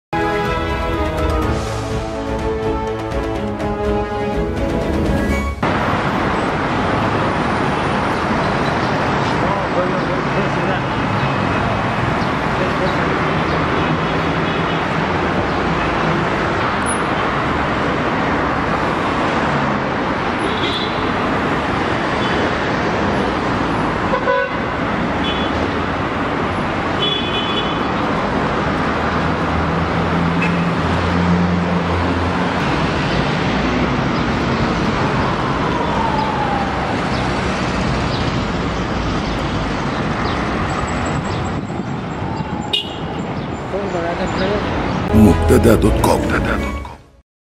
شوارع وسط البلد تتزين بأعلام فرنسا
تزين شارع قصر العينى، اليوم، السبت، بأعلام مصر وفرنسا تمهيدًا للزيارة المرتقبة لفرانسوا هولاند، رئيس فرنسا، لمصر، غدًا الأحد.